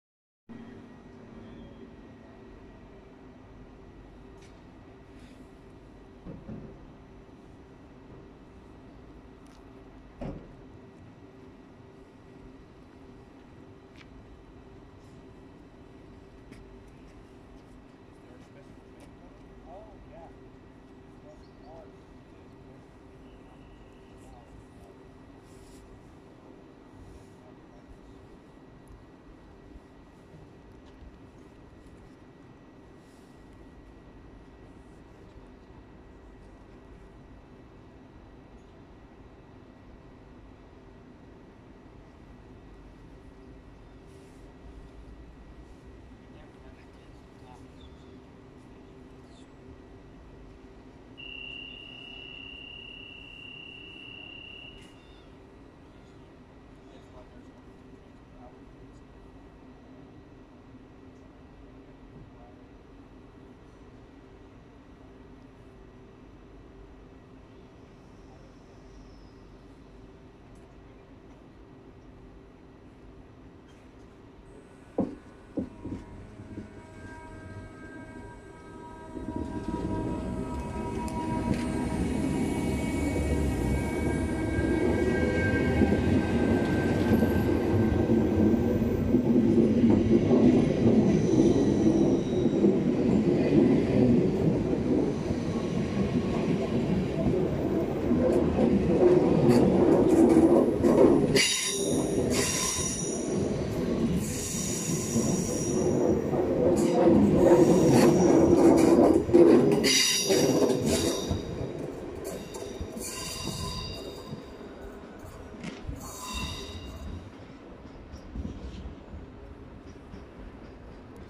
9 bakken ICMm vertrekken van Alkmaar
Op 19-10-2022 werd er wat ICMm ingezet richting Alkmaar en Den-Helder, hier het vertrek van de 4074, 4097 en 4047 als IC naar Amersfoort Centraal